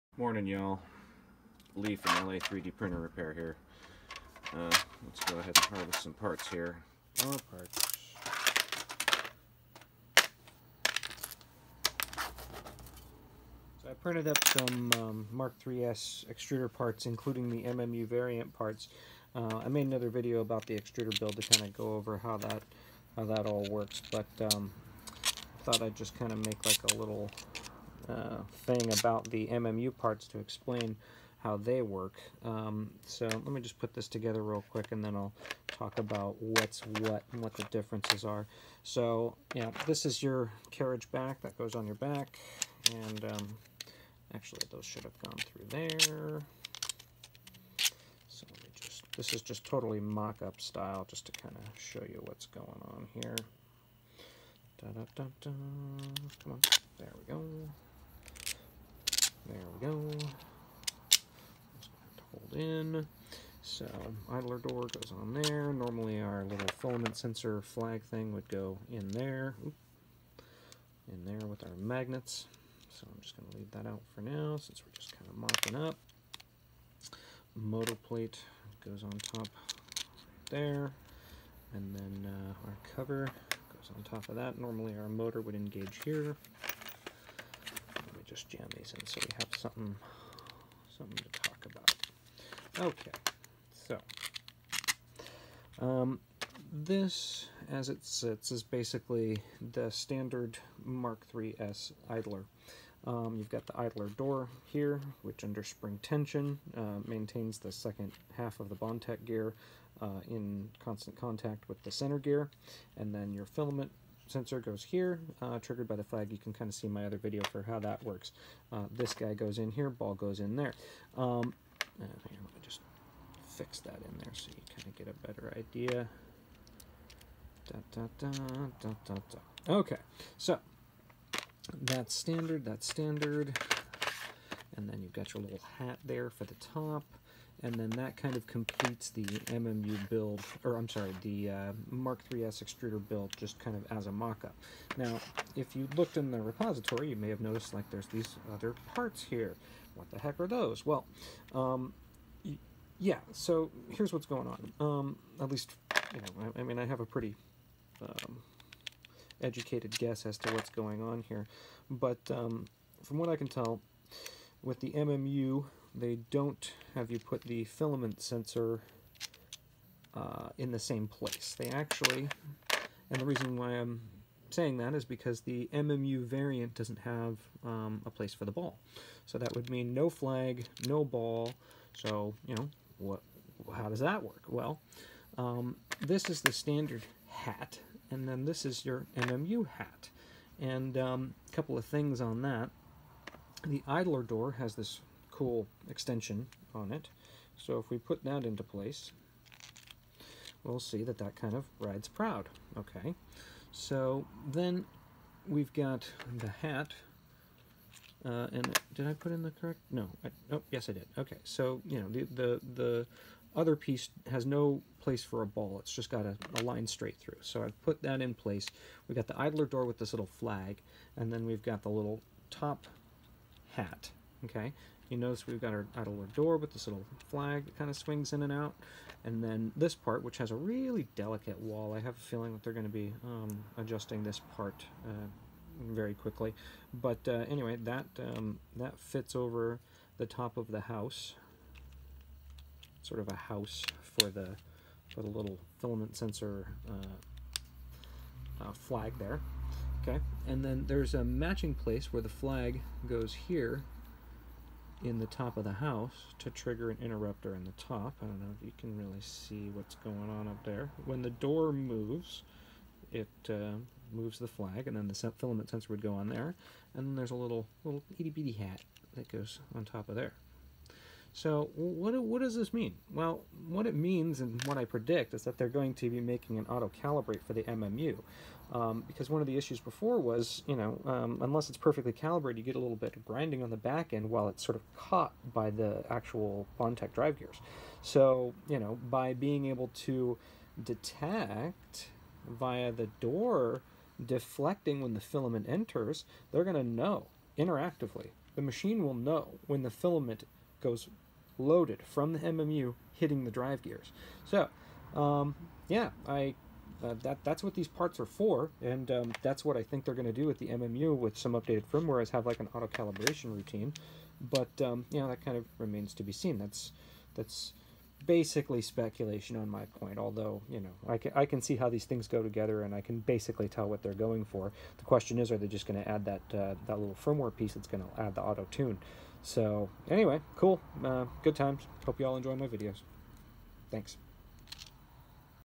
Prusa MMU2S Extruder Preview
Just a quick mockup assembly of the MMU2S Extruder variant parts included in the MK3S distribution, and speculation on new features and operational improvements.